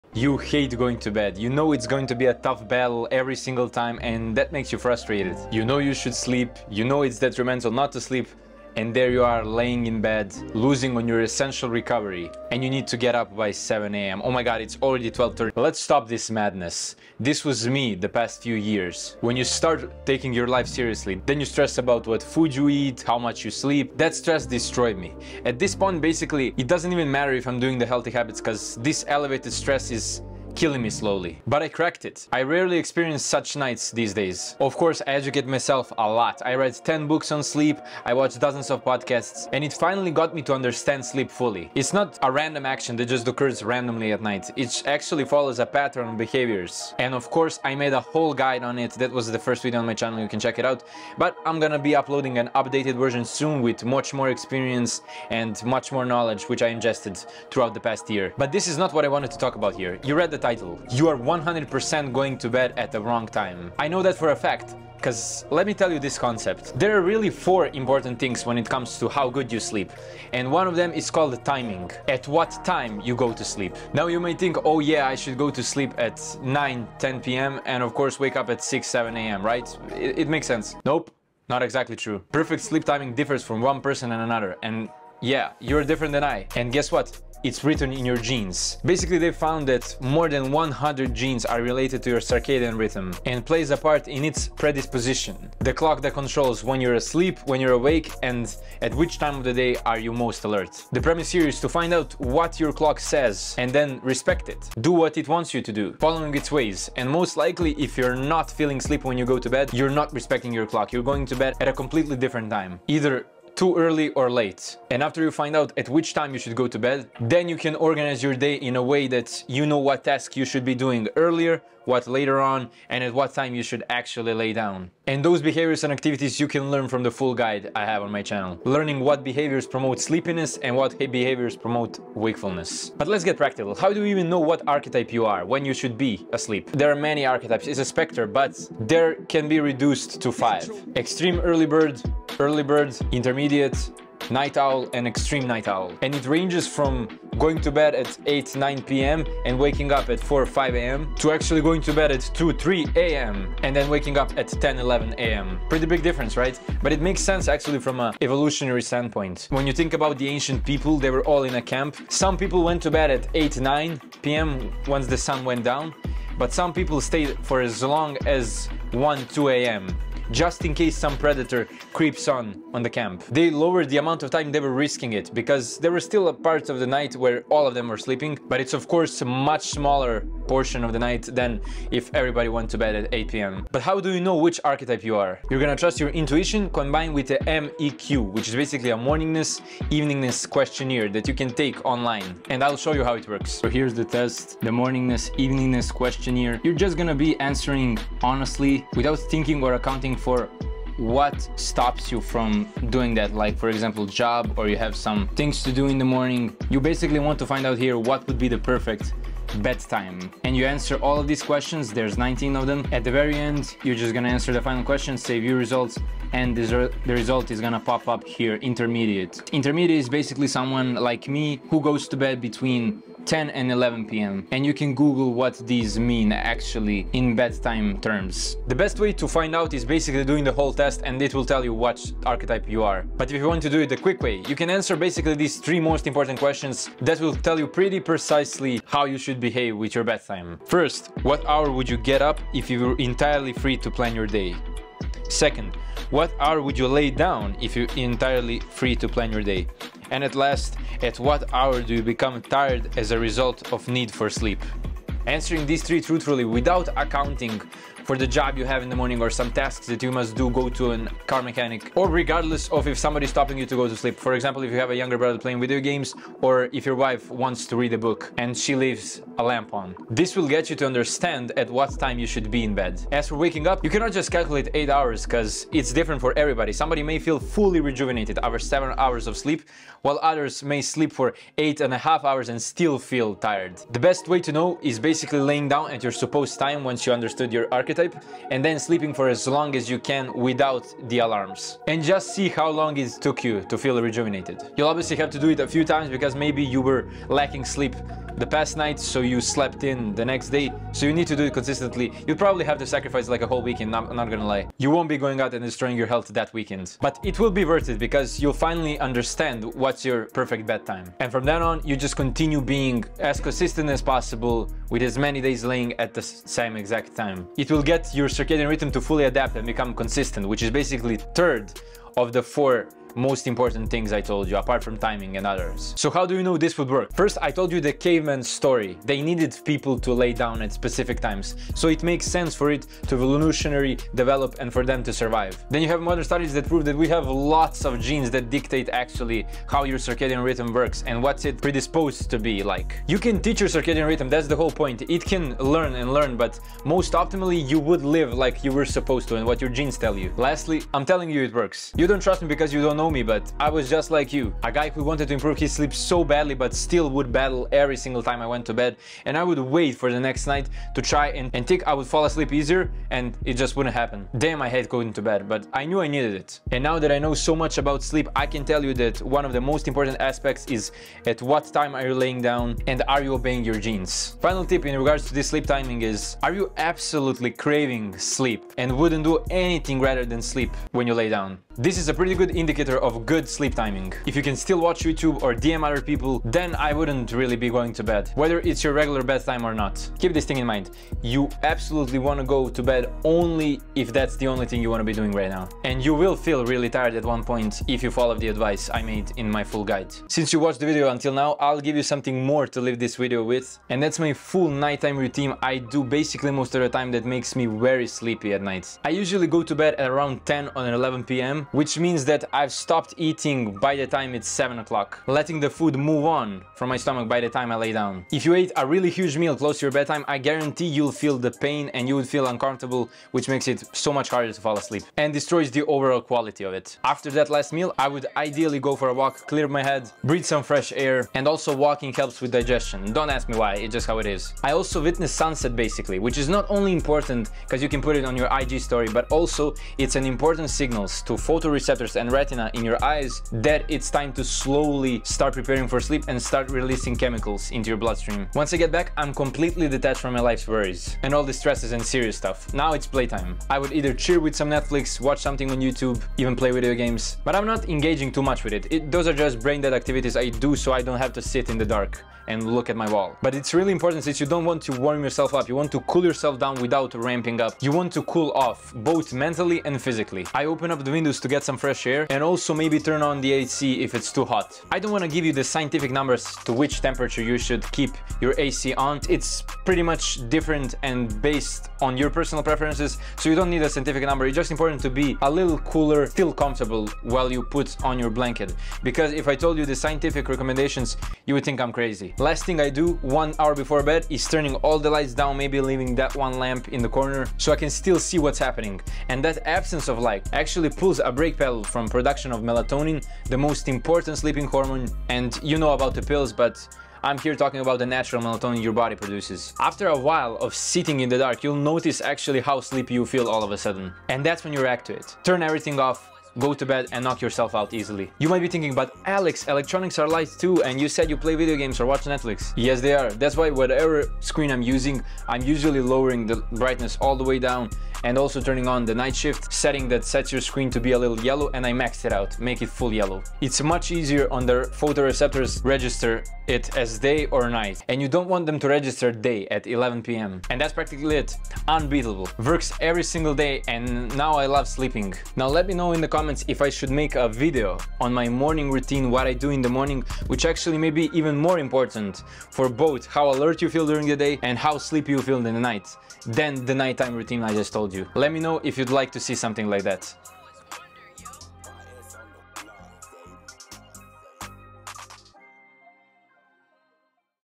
You Are 100% GOING TO BED At The WRONG TIME
Timestamps:

0:00 HOURS OF SLEEP EDUCATION LED ME HERE
1:22 PERFECT SLEEP TIMING IS PERSONAL
2:12 HOW TO FIND YOUR IDEAL BED TIME
5:25 WHY THIS WORKS
6:43 BONUS EVENING ROUTINE